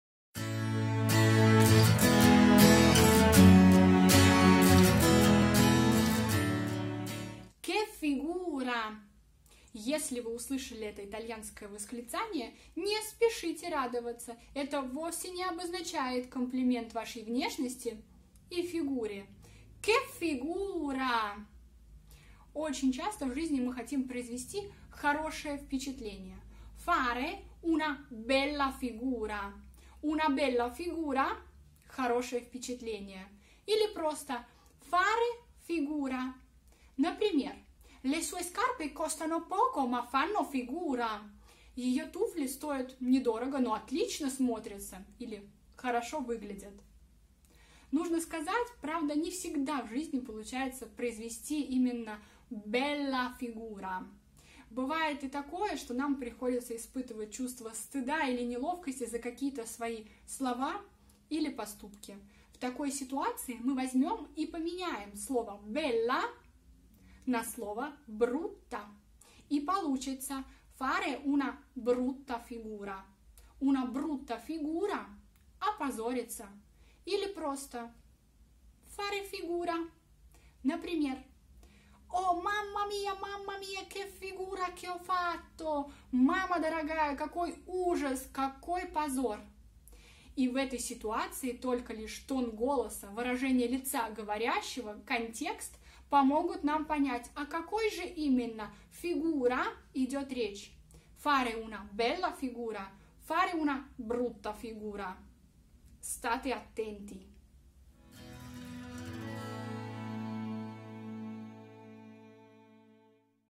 CHE FIGURA! | Полезные итальянские выражения на каждый день | Italiano per modo di dire
🙈Итальянское восклицание - Che figura! - совсем не означает комплимент вашей внешности и вашей фигуре 😉🤓⠀
📍fare una bella figura (произвести хорошее впечатление)⠀
🇮🇹Le sue scarpe costano poco, ma fanno figura.⠀
🇷🇺Её туфли стоят недорого, но они отлично на ней смотрятся!⠀
📍fare una brutta figura (произвести плохое впечатление)⠀
🇮🇹O mamma mia! Mamma mia, che figura che ho fatto.⠀
🇷🇺О, Боже! О, Боже, какой позор!⠀
Вообщем, вы поняли….. Che figura! Какой ужас! 🙈⠀